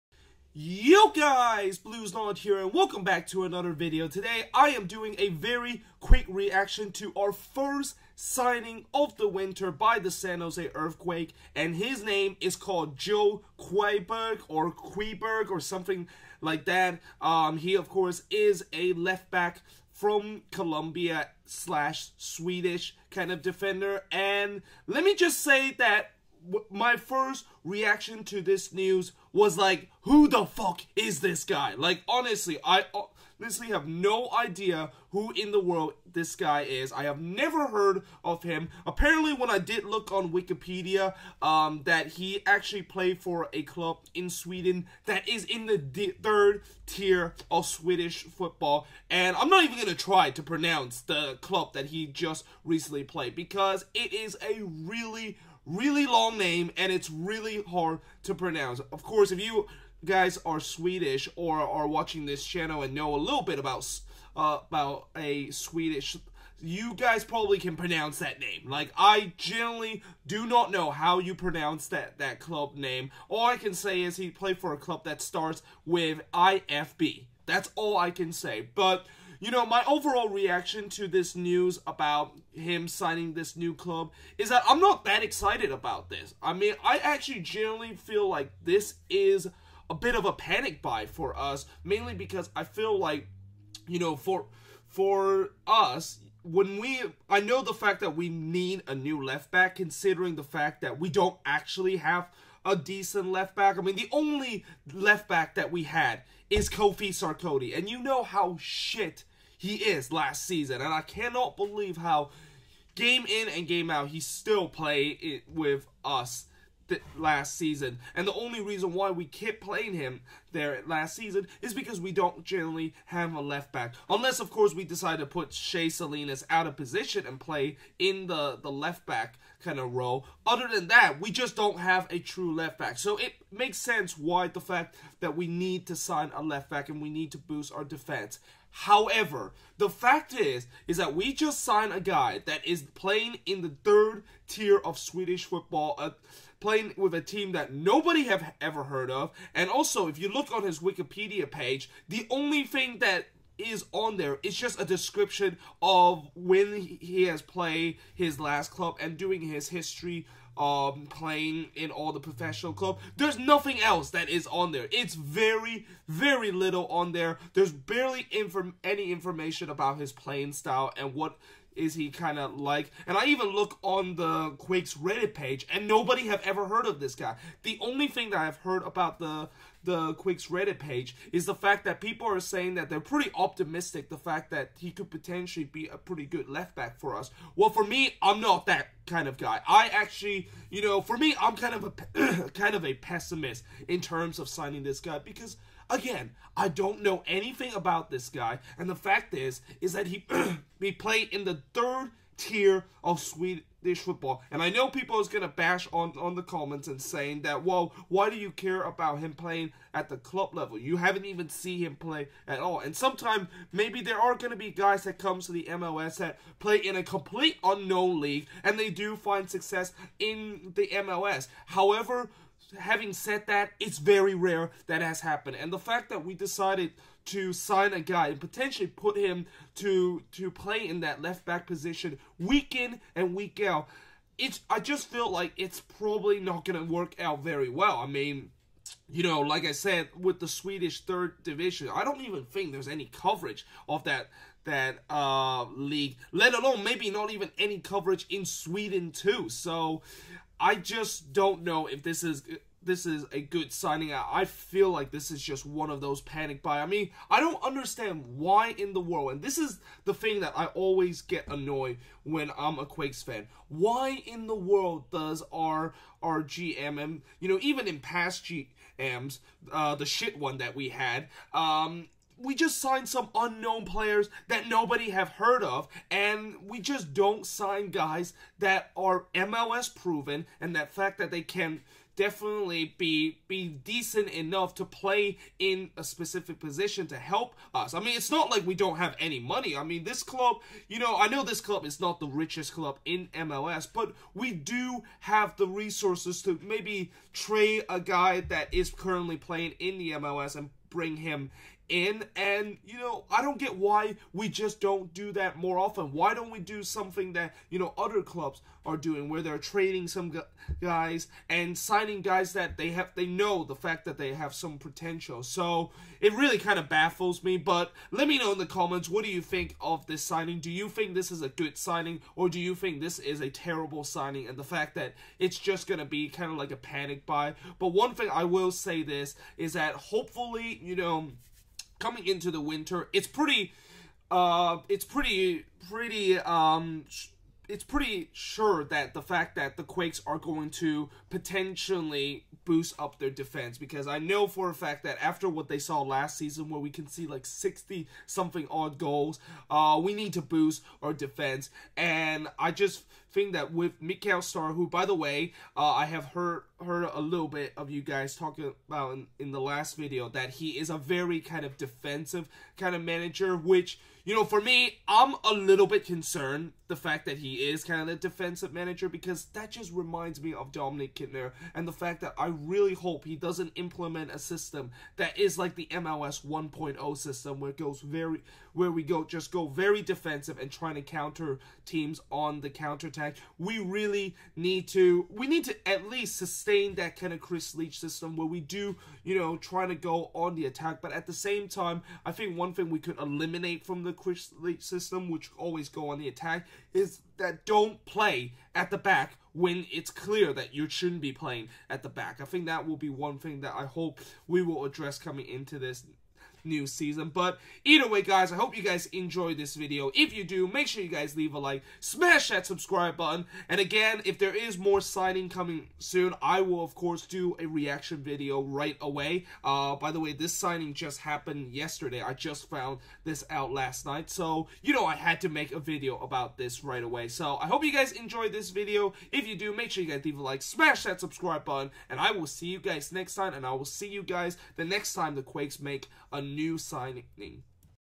San Jose Earthquakes sign left back Joel Qwiberg (My reaction)
This is my reaction to our first winter sign as we have sign a Colombian/ Swedish left back call Joel Qwiberg from a club in the Swedish third tier division.